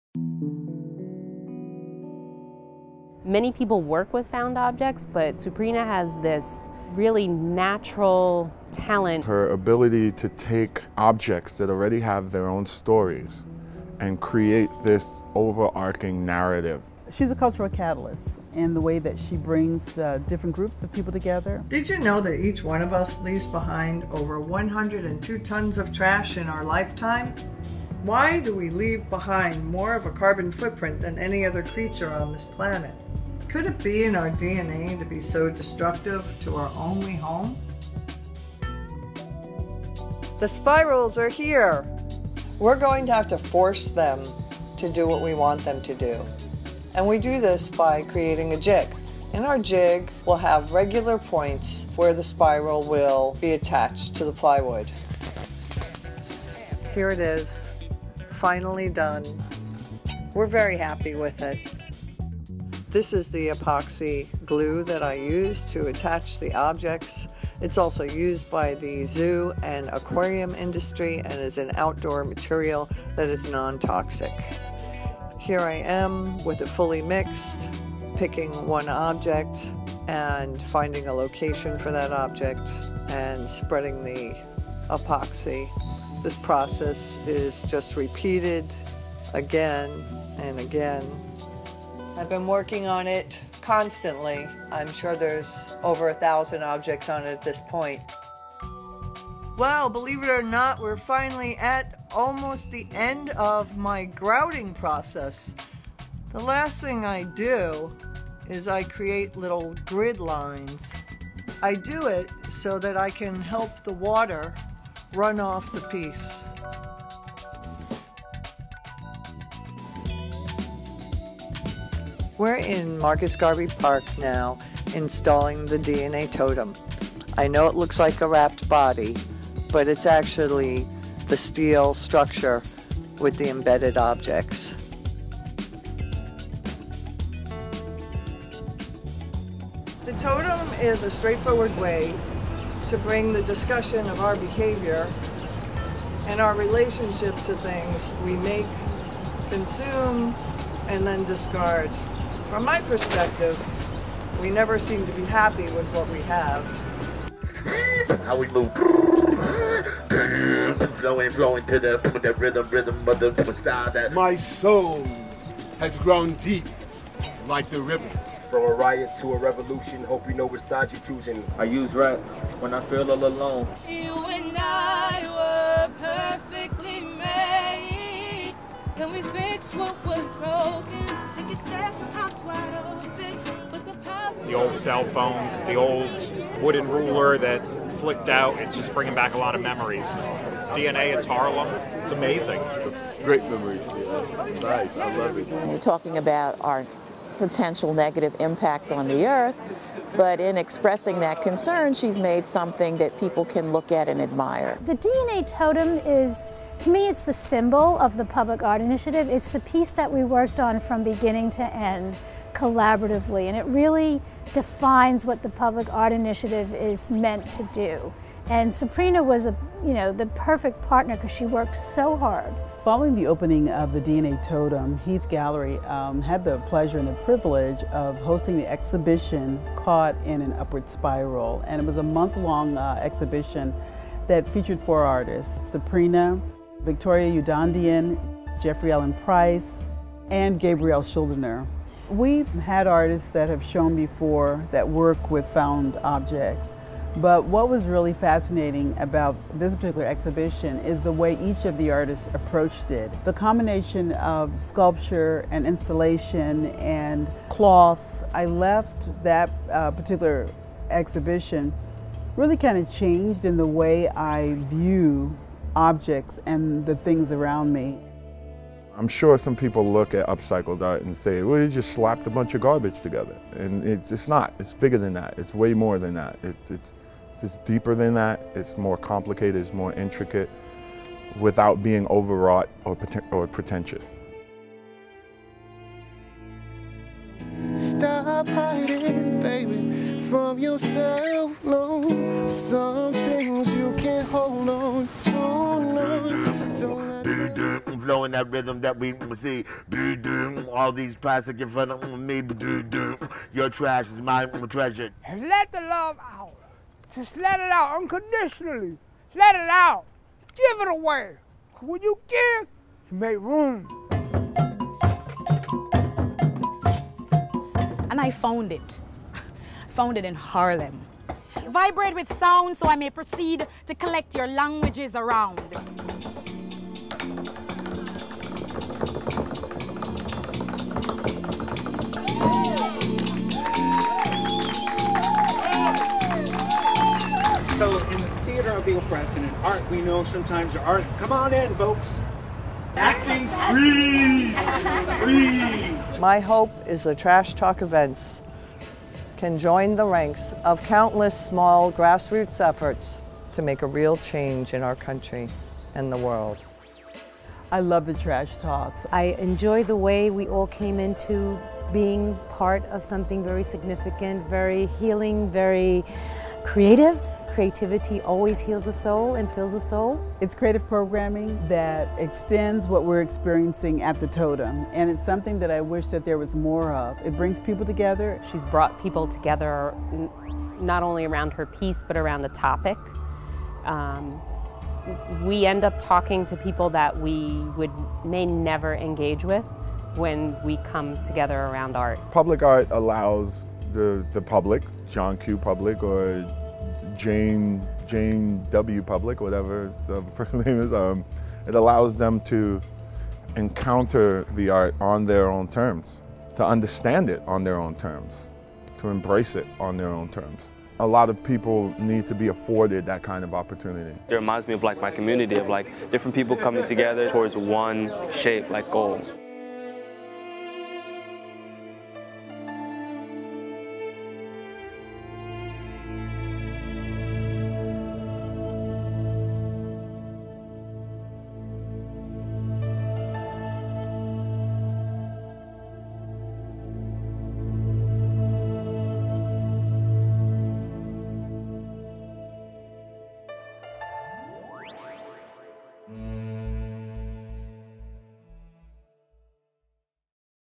DNA Totem From Start to Finish
From the first day of fabrication until the last "Trash Talk" event. This video documents the process of public sculpture. Video created by Peter Cooper.